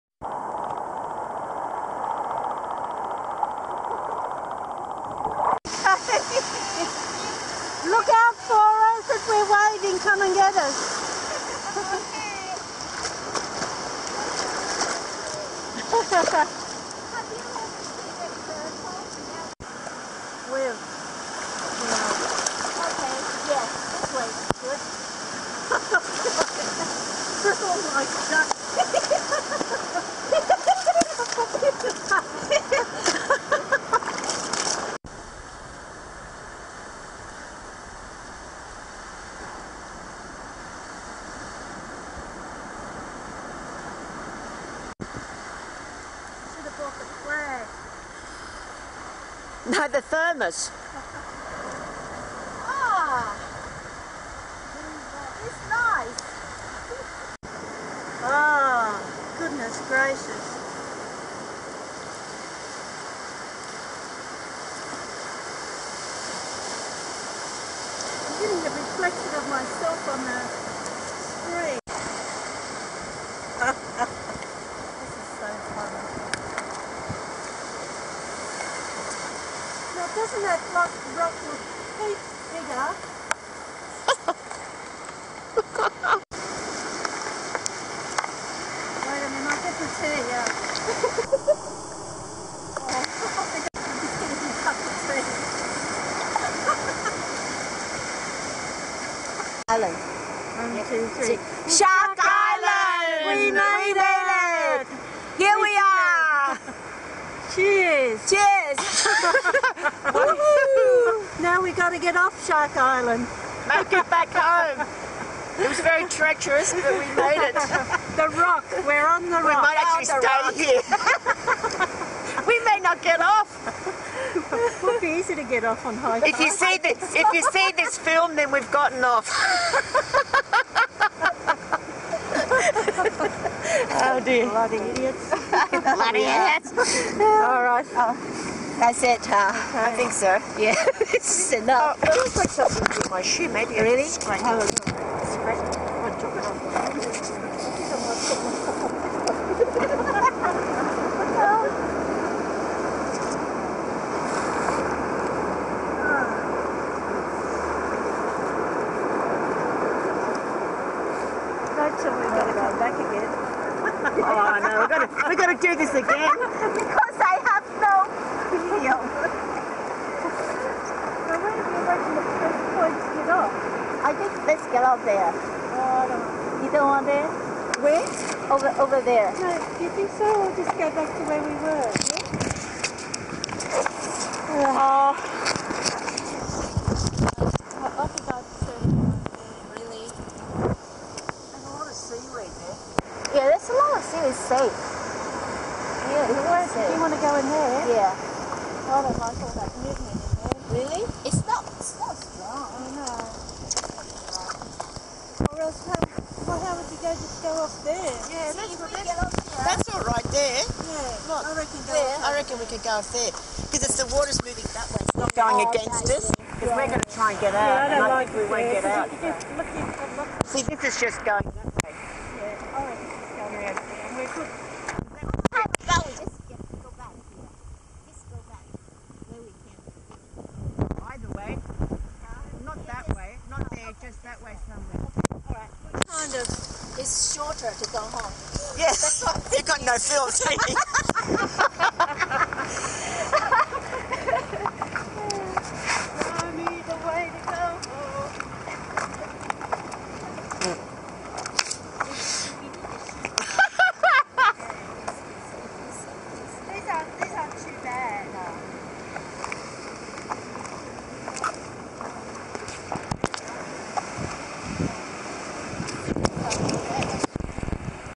Landing on Shark Island
Pam, Diana and myself (Lucy) finally landed on Shark Island to enjoy the view and a cup of tea!